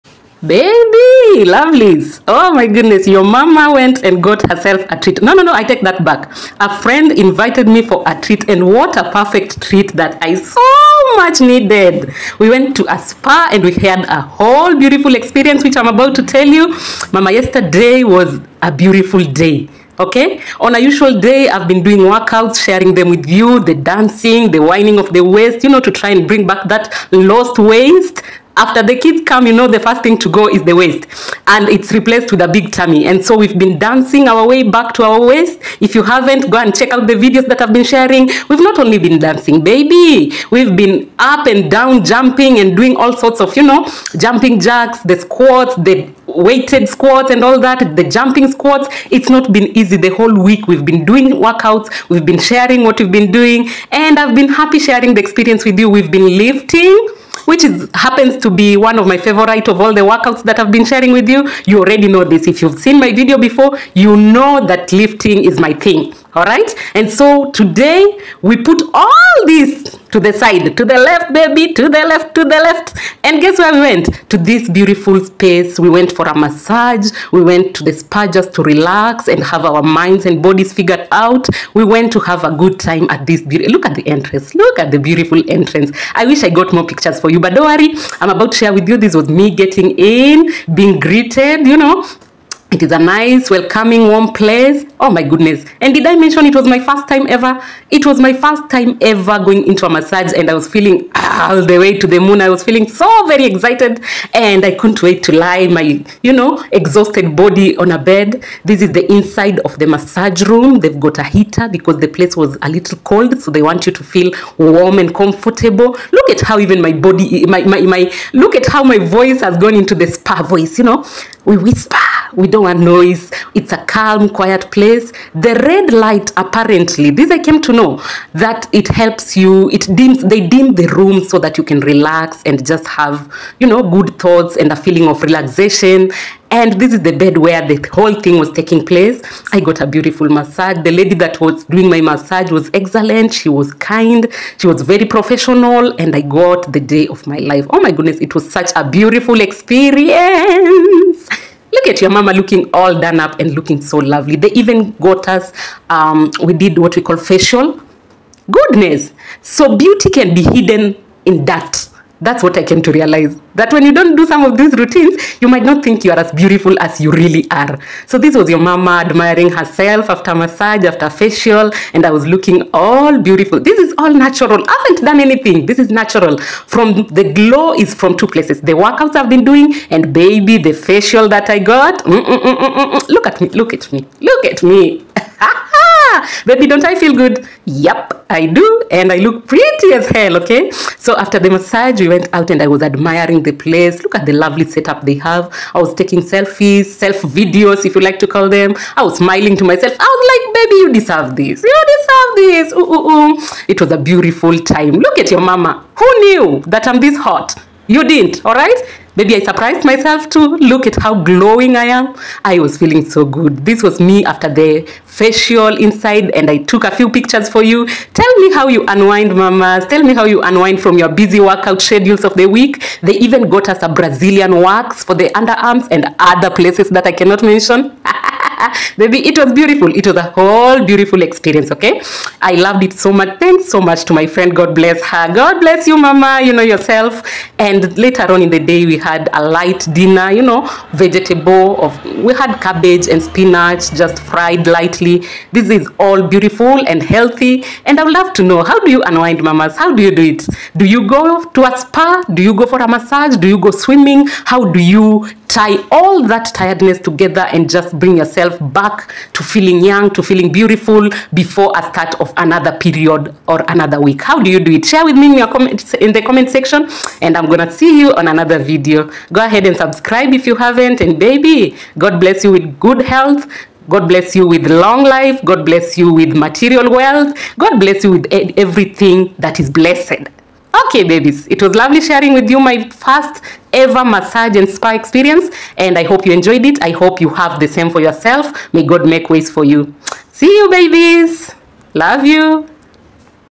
My Spa Treat Experience 🥰 (Lose Weight With Ronji)
Hey y'all lovelies,
Today I share with y'all my beautiful 1st ever spa treat experience on our favourite series Lose Weight with Ronji.

We workout too hard not to enjoy these kinds of healthy treats.
some of the benefits of Spa massage include; mind and body relaxation, rest, feel good hormones elevated, good moods and so much more.

Even as we Lose Weight With Ronji, find time and spare some cash and treat yourself to a beautiful massage once in a while.